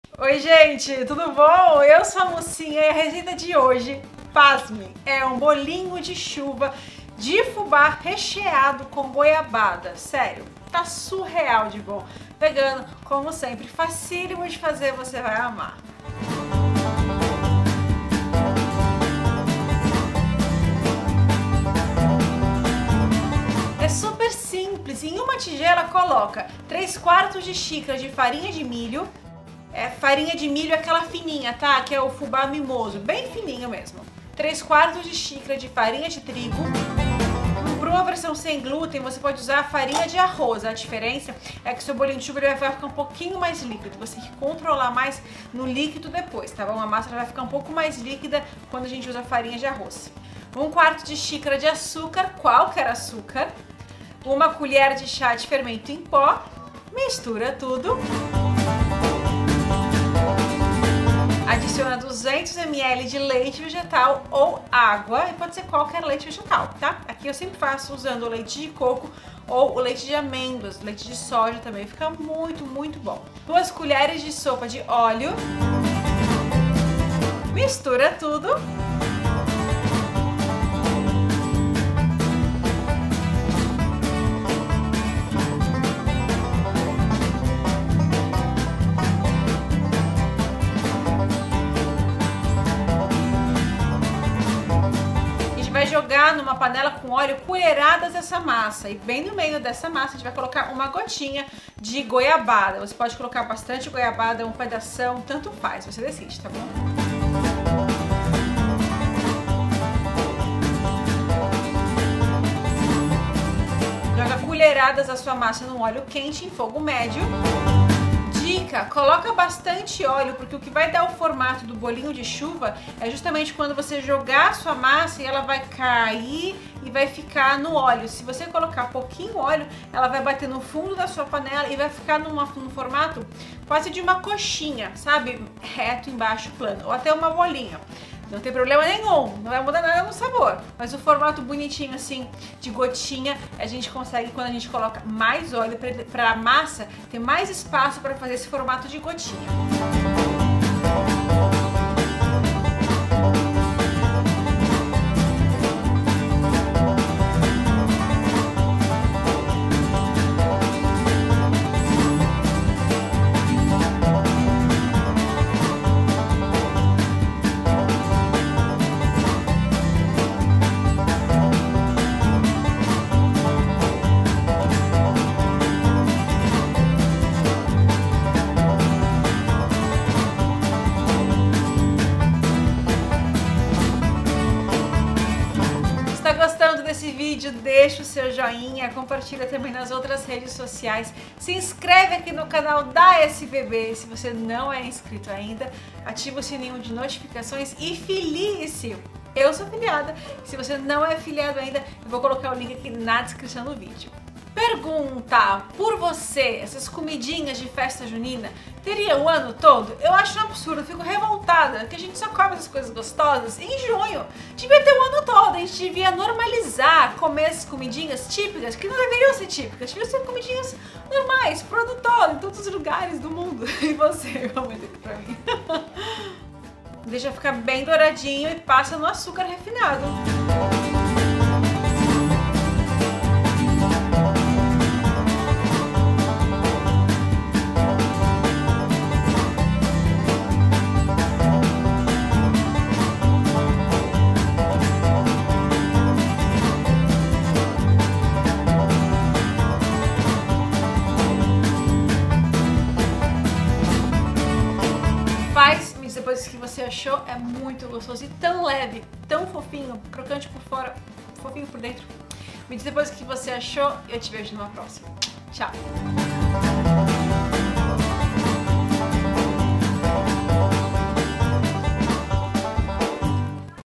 BOLINHO DE CHUVA DE FUBÁ COM GOIABADA (SEM LACTOSE/SEM OVO) #63
Facebook.com/SociedadeVegetarianaBrasileira

Bolinho de Chuva de Fubá com Goiabada!

Ingredientes:
3/4 xic. farinha de milho (fubá mimoso)
3/4 xic. farinha de trigo (para versão sem glúten, use 1/2 xic. Farinha de arroz + 1/4 xic. Polvilho doce ou amido de milho)
1/4 xic. Açúcar
1 c.c. Fermento em pó
200ml Leite vegetal ou água
2 C.S. Óleo

Goiabada em cubos!

Como fazer? Assista o vídeo completo para ver o passo a passo!